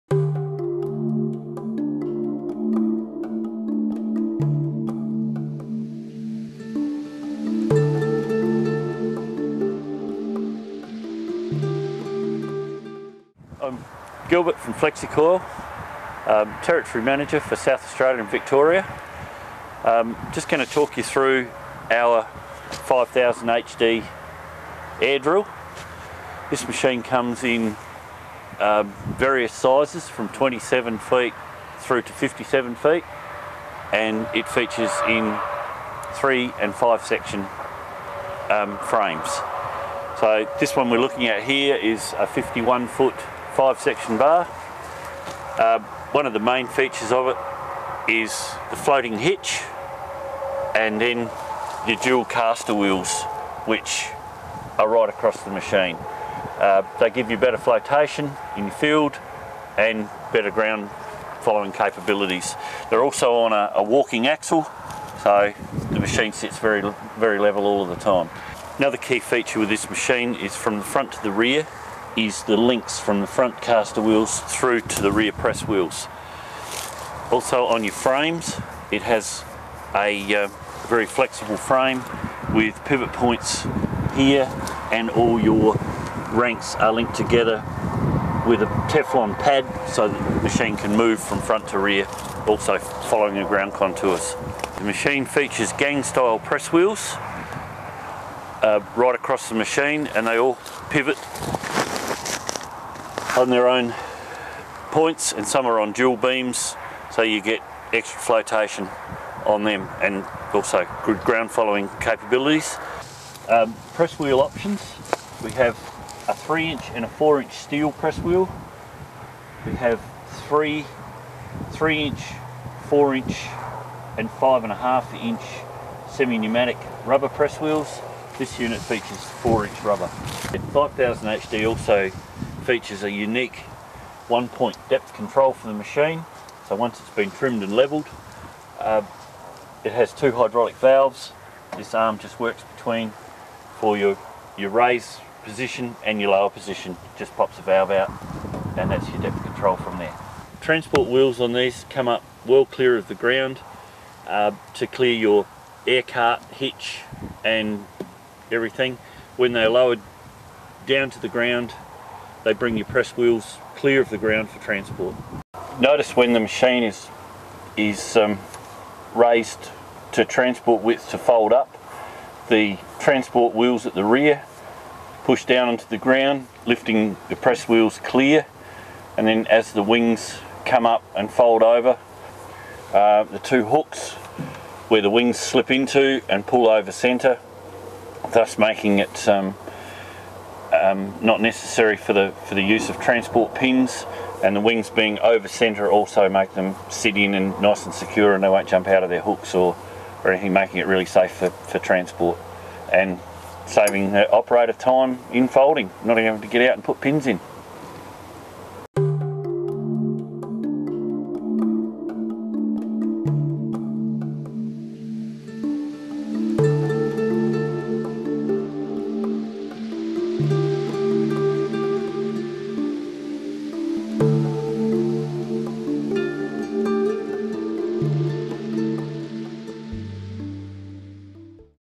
Flexi-Coil 5000HD
The 5000HD air drill lets you put seed and fertiliser in the right place for faster, more even germination. It delivers accurate seed placement with on row packing. With a flexible frame and four bar configuration, the 5000HD brings precision to a wide variety of seeding conditions. Available in widths from 12 m (40') (three-section), and 13.76 m (45') to 17.37 m (57') (five-section).